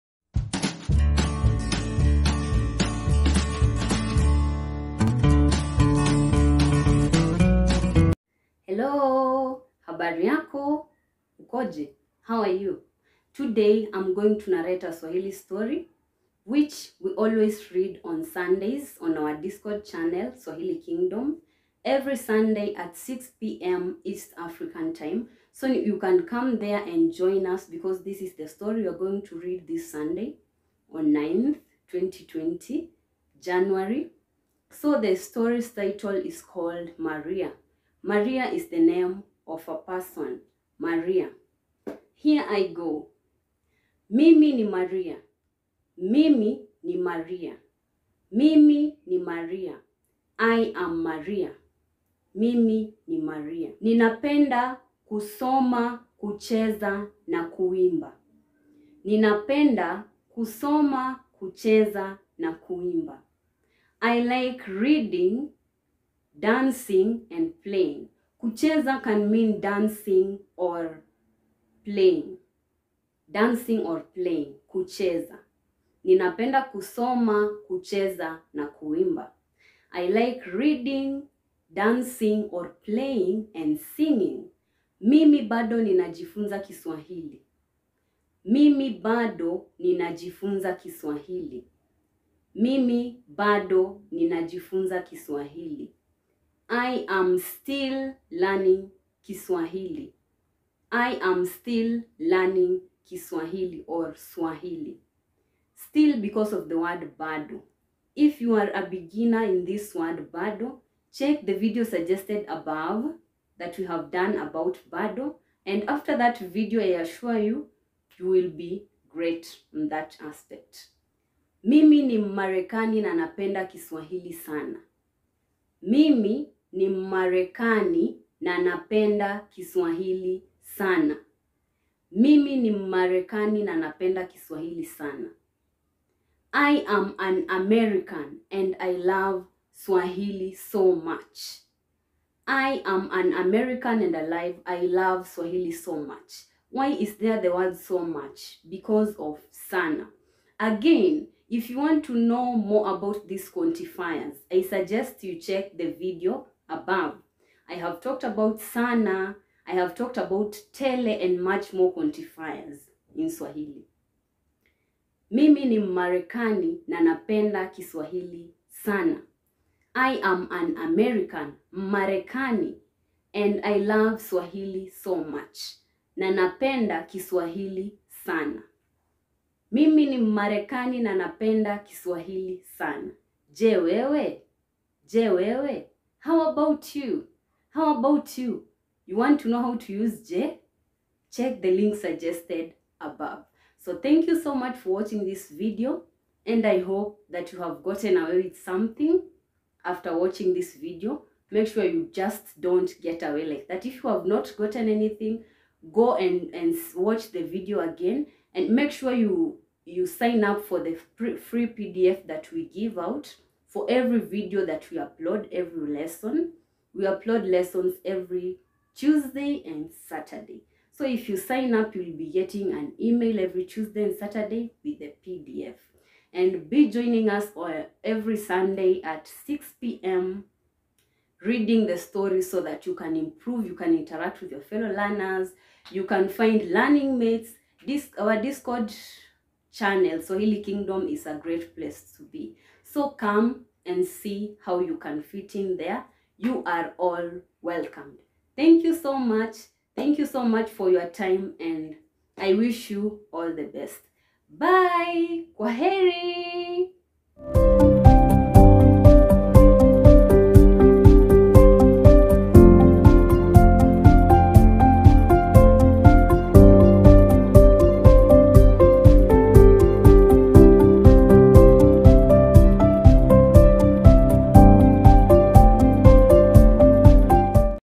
HOW TO INTRODUCE YOURSELF IN SWAHILI | A STORY SAMPLE FOR BEGINNERS
How to INTRODUCE YOURSELF IN SWAHILI |  A STORY SAMPLE FOR BEGINNERS . Use a real example to stand out in the crowd with Introducing Yourself. Good luck! Kila la heri!

All the best in your learning . Enjoy!

✔️Join our other Social media accounts here

✔️ INCREDIBLE SWAHILI GOODIES
ITALKI OFFER: BUY 1 Lesson, get 10$ Free Credits at italki

● Timestamps
00:00 Intro
00:08 What's in this video
00:41 Story
03:14 THANKS
04:18 Outro